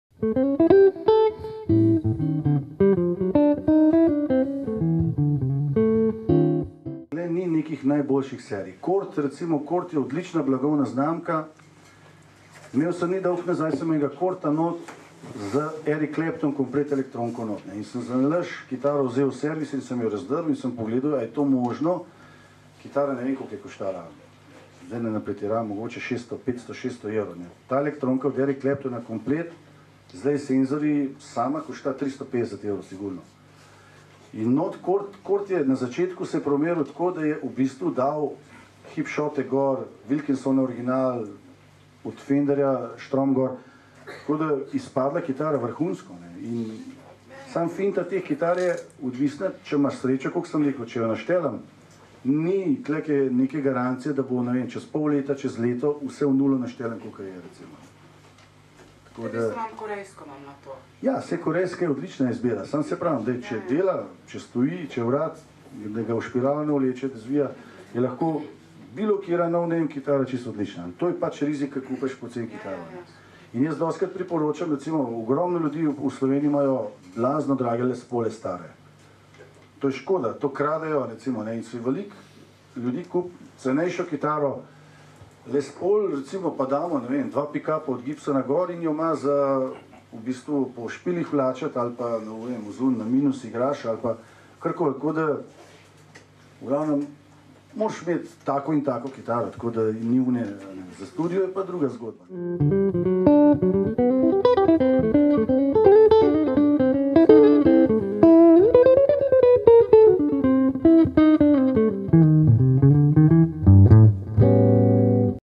Nasveti za nakup kitare | O različnih znamkah kitar | Tomaž Šinko
Prav tako lahko vidite, kako zaigrati pesem. Video vam pomaga pri učenju kitare.